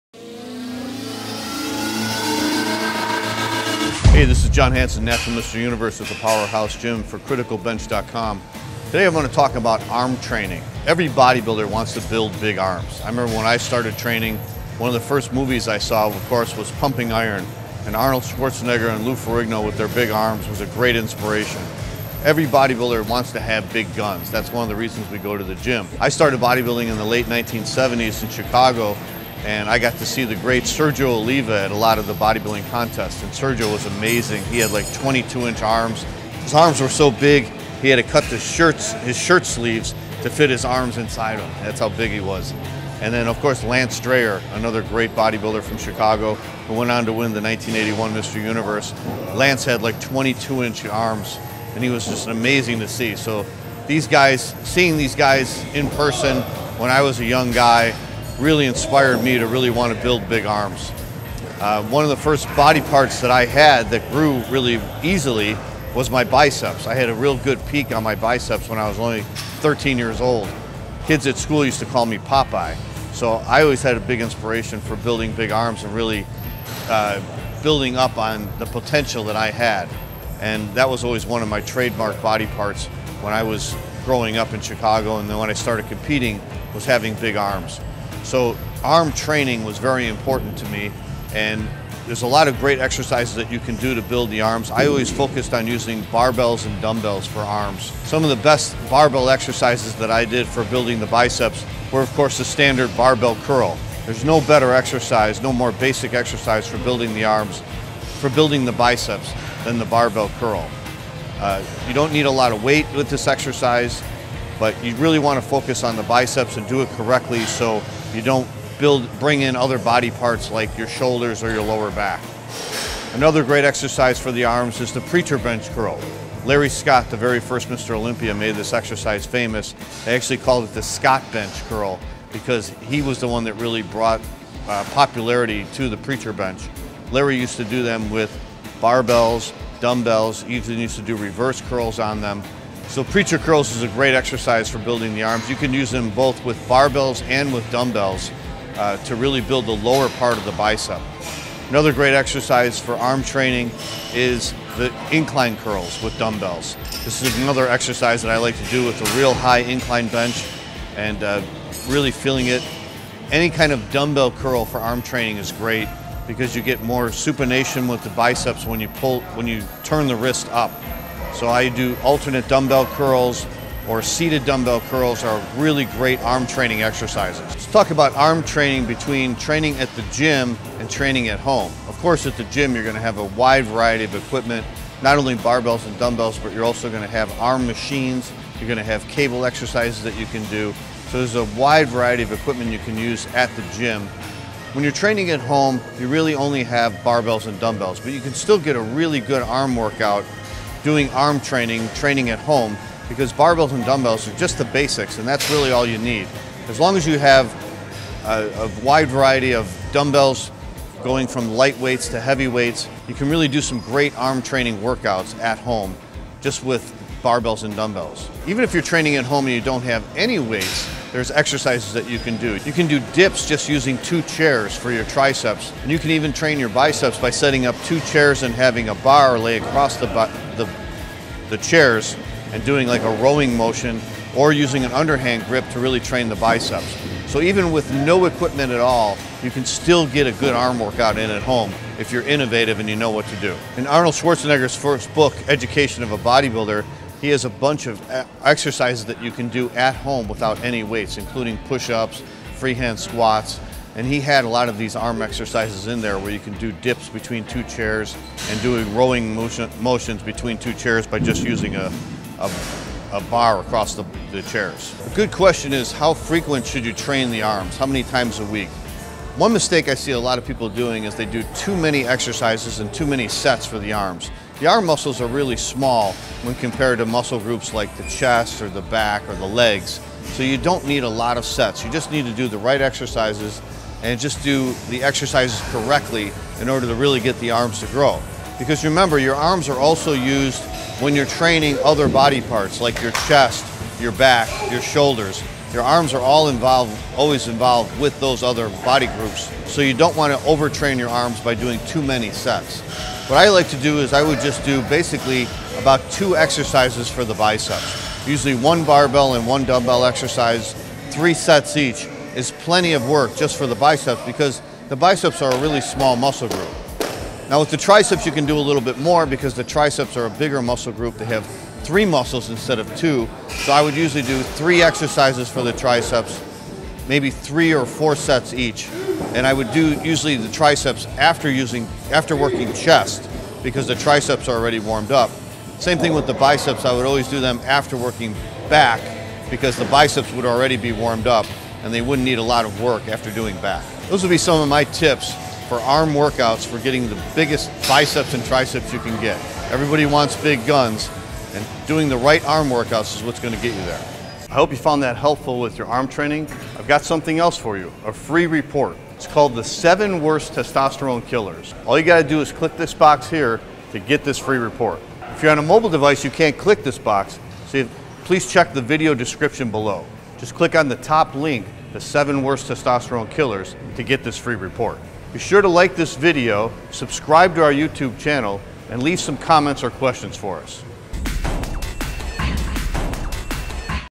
Arm Workout Tips for Men to Build Mass at Gym or Home
Natural Champion John Hansen (3x Natural Mr Universe & first ever Natural Mr Olympia) talks about arm training at home or the gym to get the biggest biceps and triceps.  John is a bodybuilding historian and shares some amazing insights about the best bodybuilders in the last 40+ years!

7 FASTEST WAYS to Increase Your Bench - FREE report

5 Ways to Become Tough As Nails - Free Report